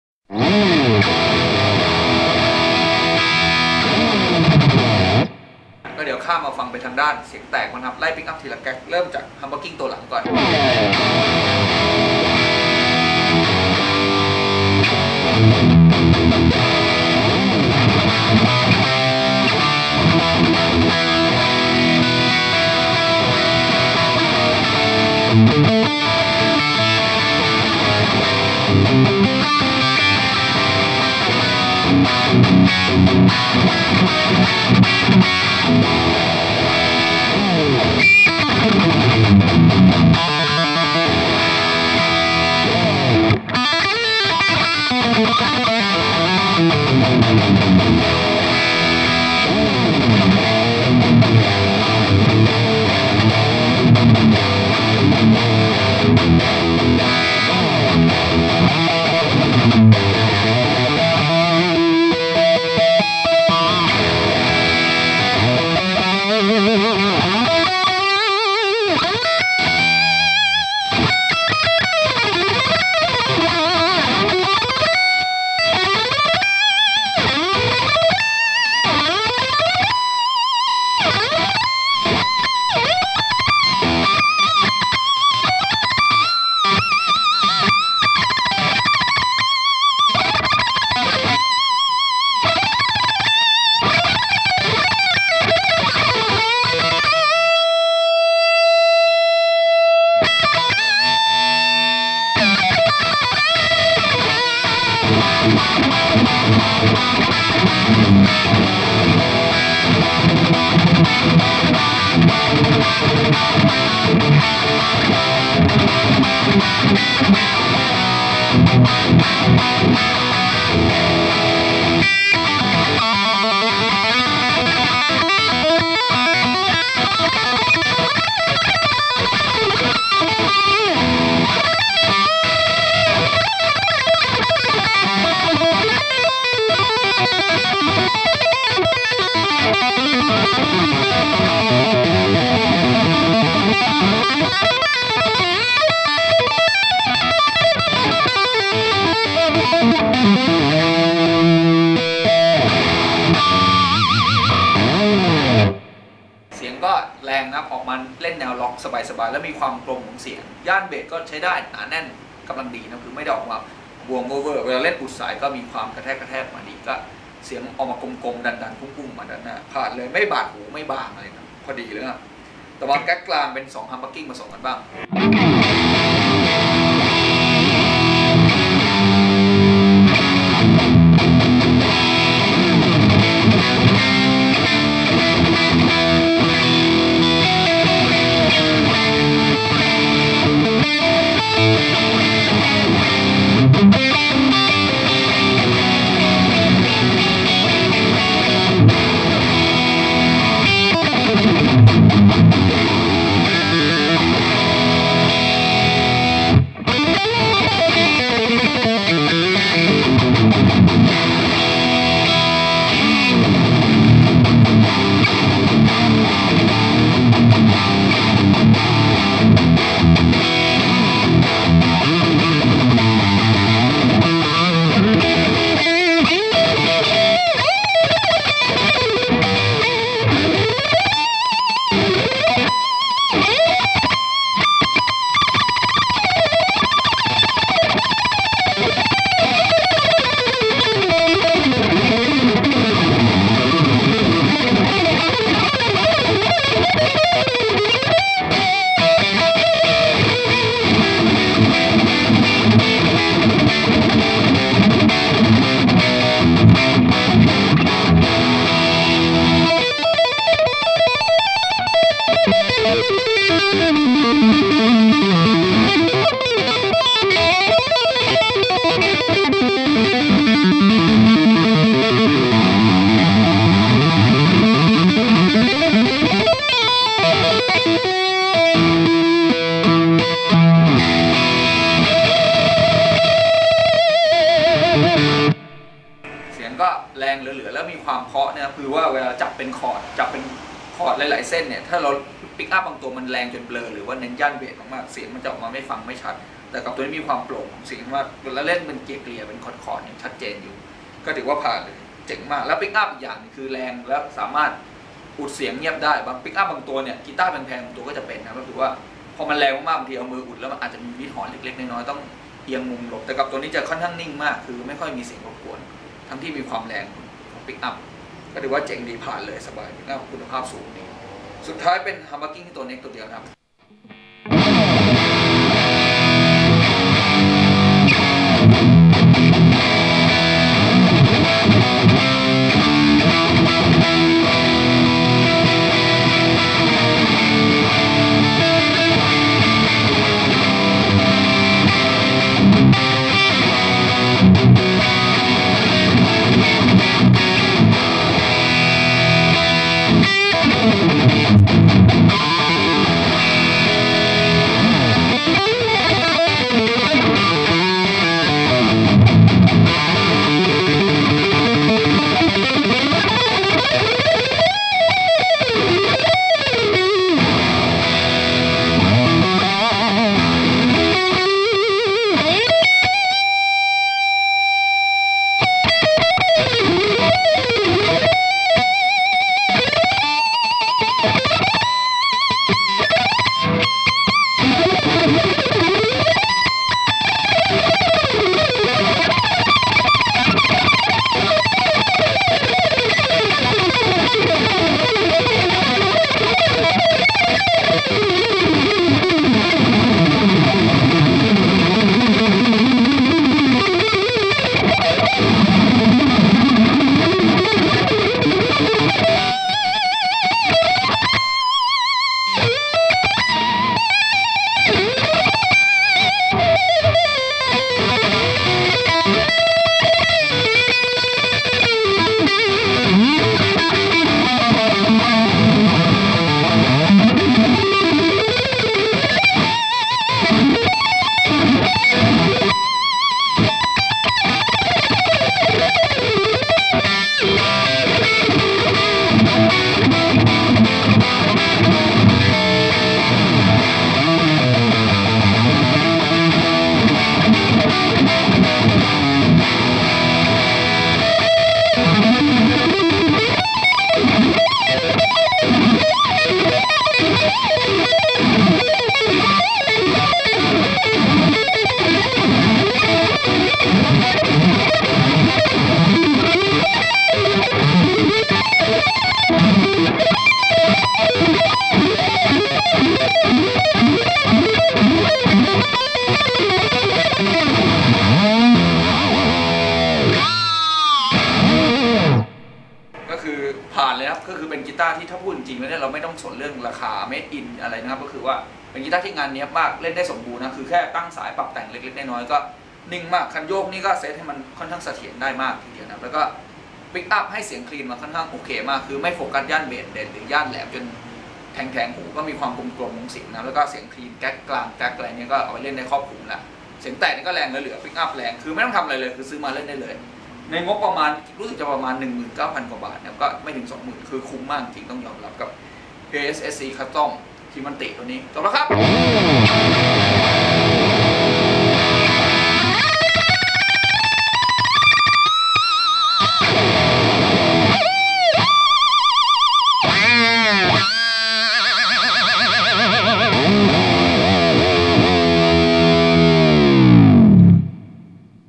Prs SE Mark Tremonti Custom 2014 Guitar Drive Sound
Prs SE Mark Tremonti Custom 2014 Guitar

DIEZEL VH4 AMPLIFIER

ENGL 4X12 STANDARD CABINET CELESTION V60 SPEAKER

PLANET WAVES AMERICAN STAGE CABLE

JIM DUNLOP JAZZ III PICK

LINE 6 PODXT LIVE USE FOR REVERB DELAY EFFECT ONLY